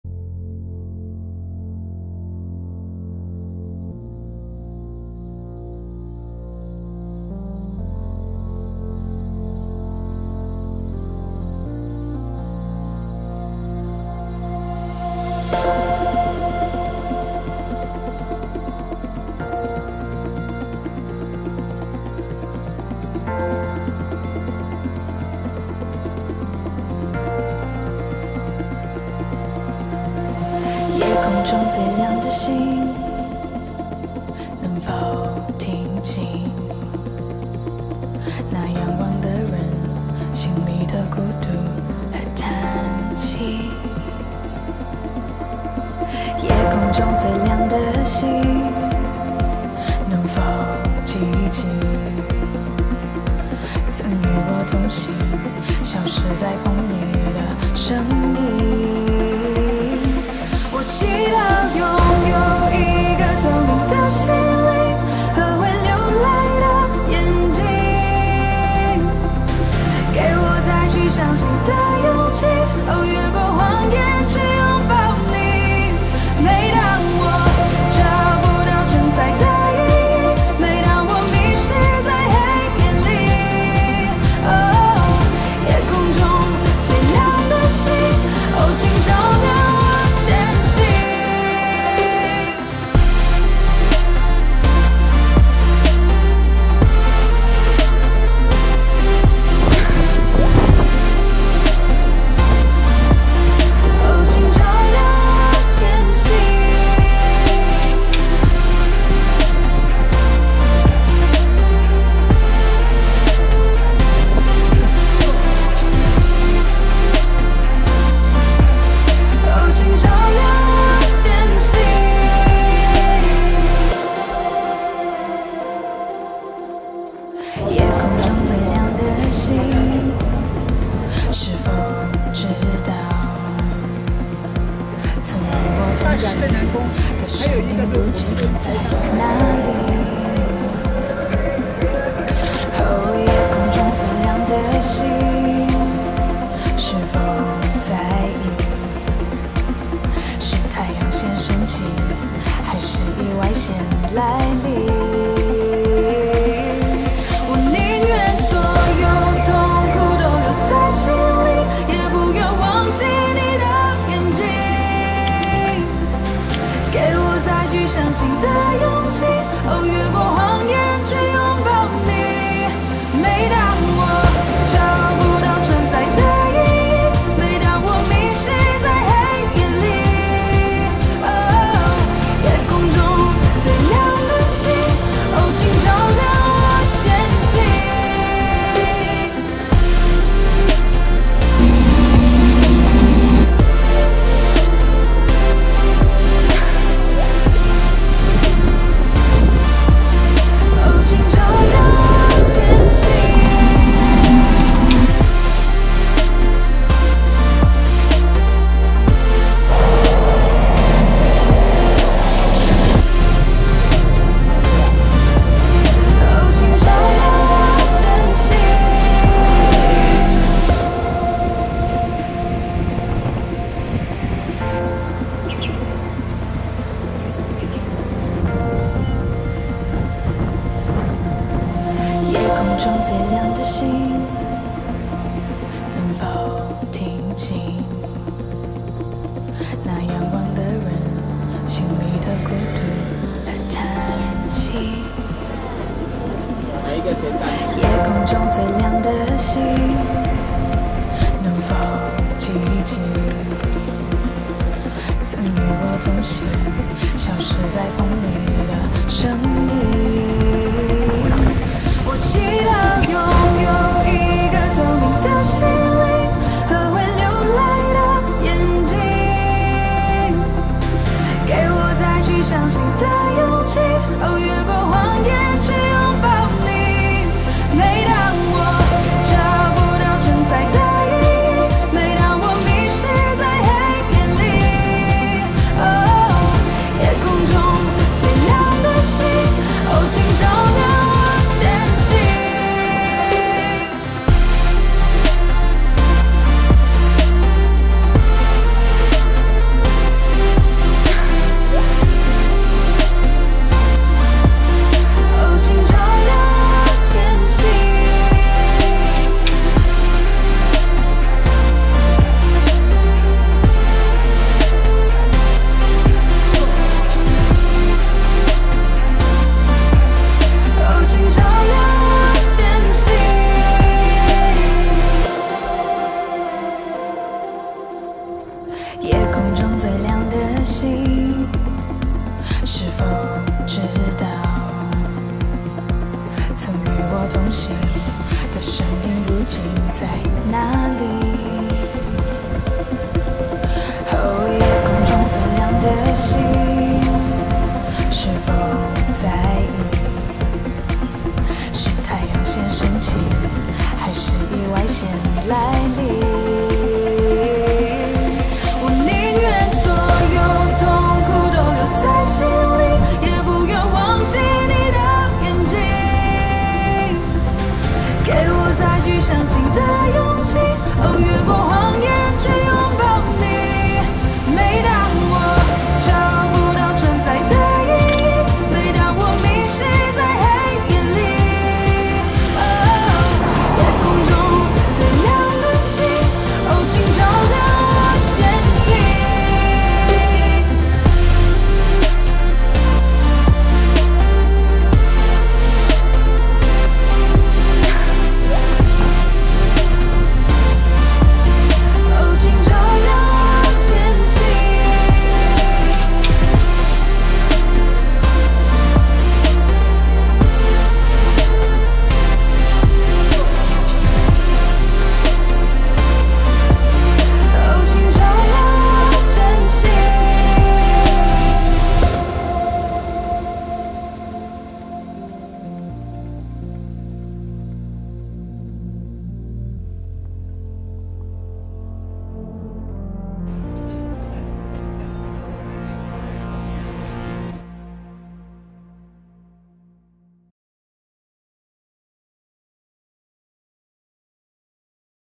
2019 Taiwan Cycling | Taoyuan to Kaohsiung
420km in 5 days from Taoyuan to Kaohsiung condensed in a video to capture the best memories.